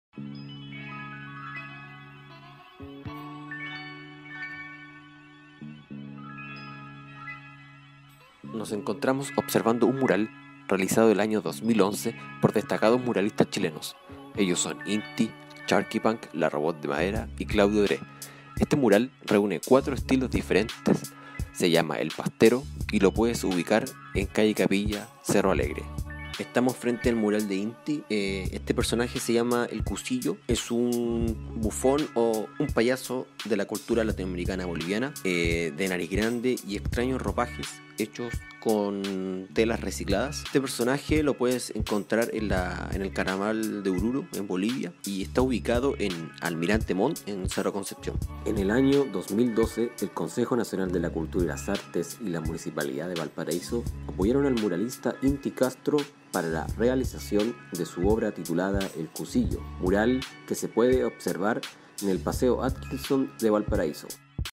La Ruta de Inti Castro
Especial de Inti Castro en Valparaiso
Valpostreetart
Difusión de arte urbano
Producción y Edición:  Nicolas Bastian
Voz en Off: Juan Francisco Astorga